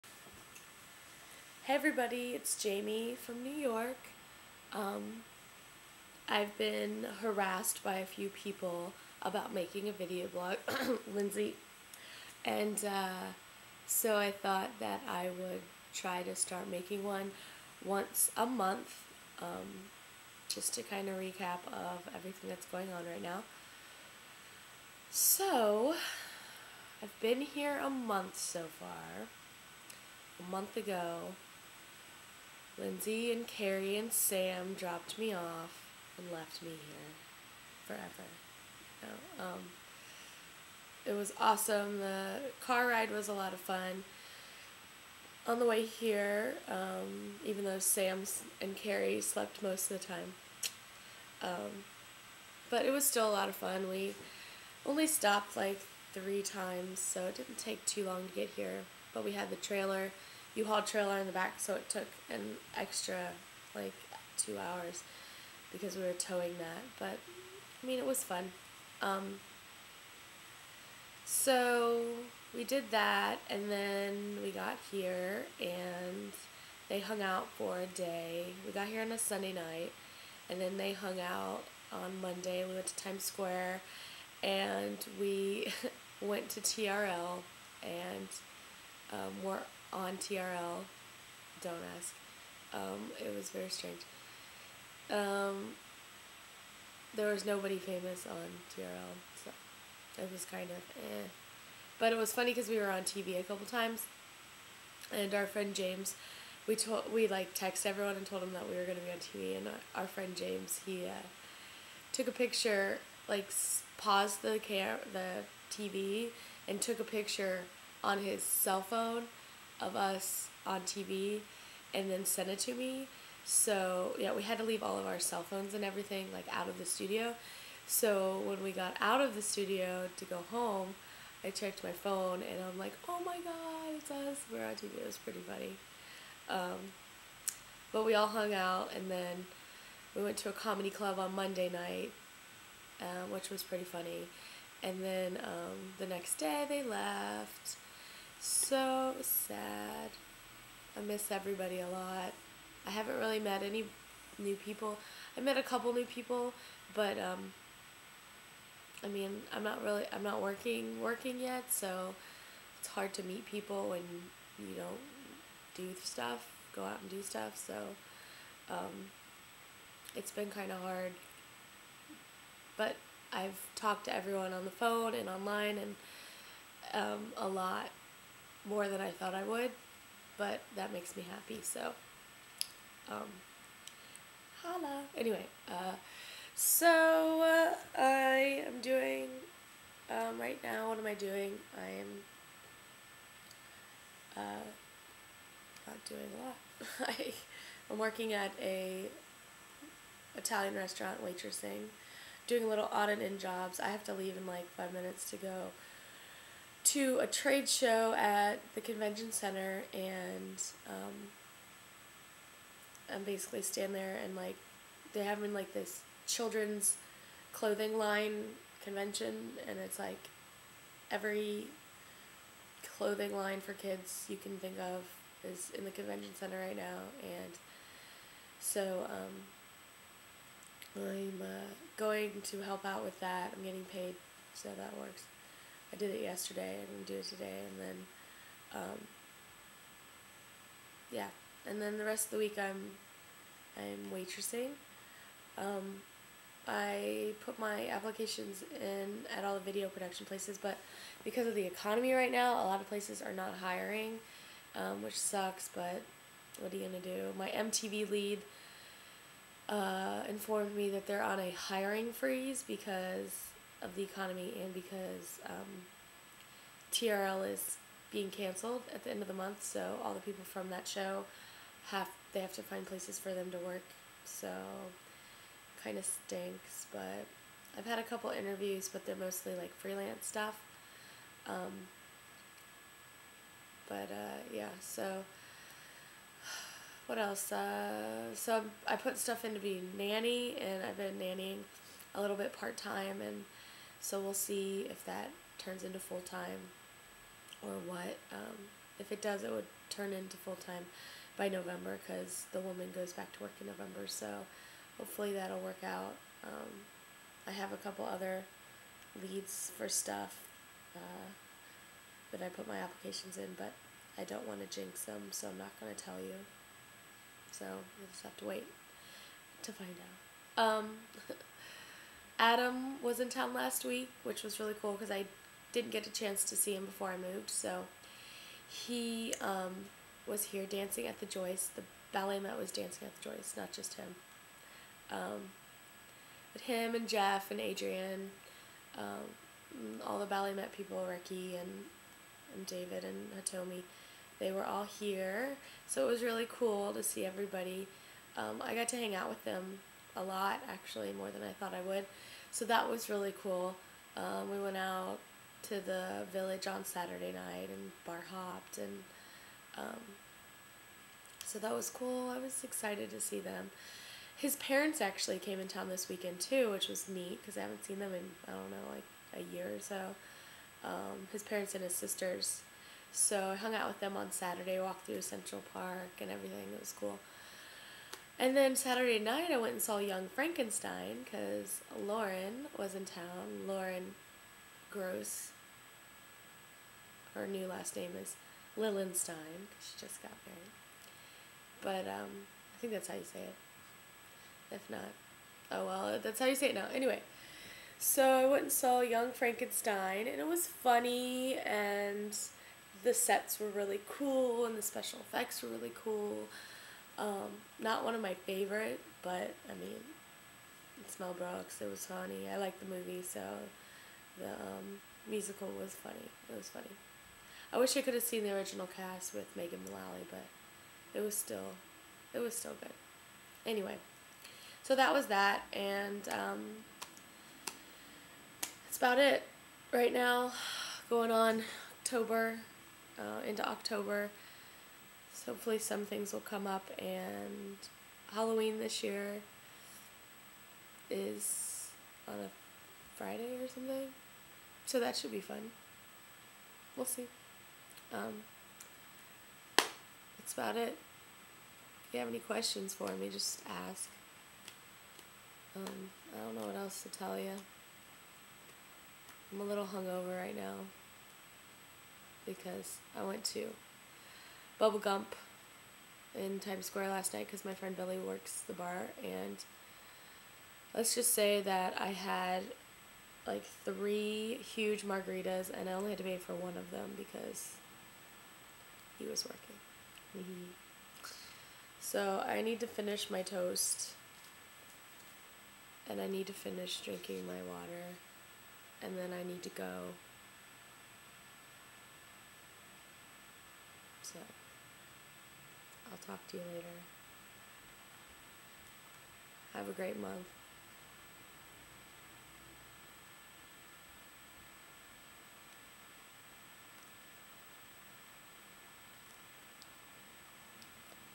Jamie's Video Blog: October 2008
MY first month in NYC... here's an update